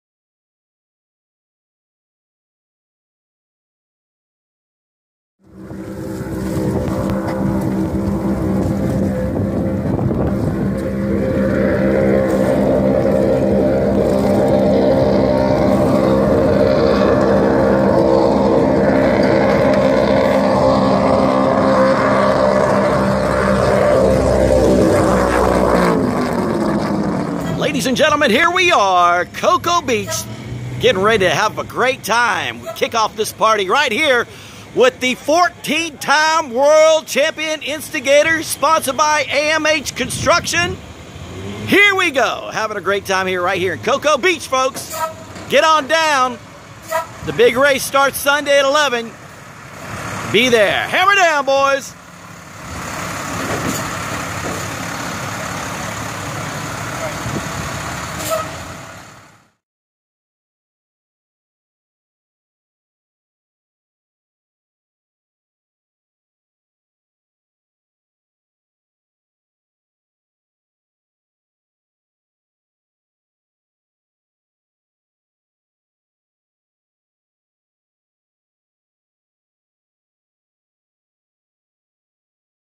AMH Construction Instigator Offshore Racing Team
The World Championship Instigator Offshore Racing Team owned by Peter & Julie Meyer welcomes Mr. Aaron Hope from AMH Construction on May 3rd , 2016 . Watch how the Instigator Racing boat gets an new World Championship look as it takes on  all competitors and takes the WINNING flag in our first race of the season at Thunder on Cocoa Beach .